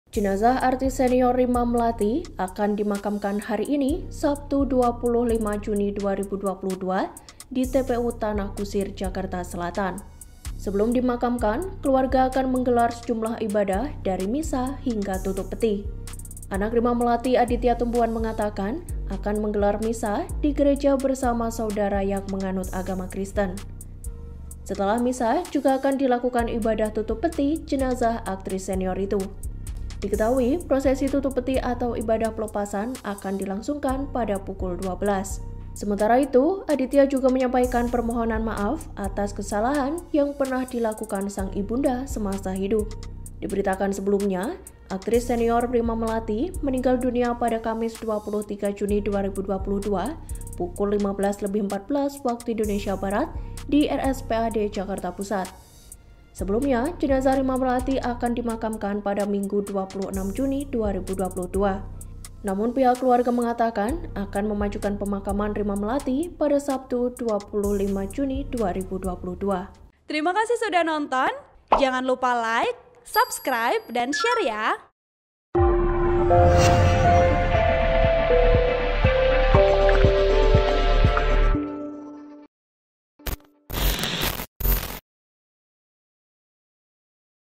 Jenazah Rima Melati Dimakamkan Hari Ini, Keluarga Lakukan Misa hingga Tutup Peti
TRIBUN-VIDEO.COM - Jenazah artis senior Rima Melati akan dimakamkan hari ini, Sabtu (25/6/2022) di TPU Tanah Kusir, Jakarta Selatan.

Sebelum dimakamkan, keluarga akan menggelar sejumlah ibadah dari misa hingga tutup peti.

Anak Rima Melati, Aditya Tumbuan mengatakan akan menggelar misa di gereja bersama saudara yang menganut agama Kristen.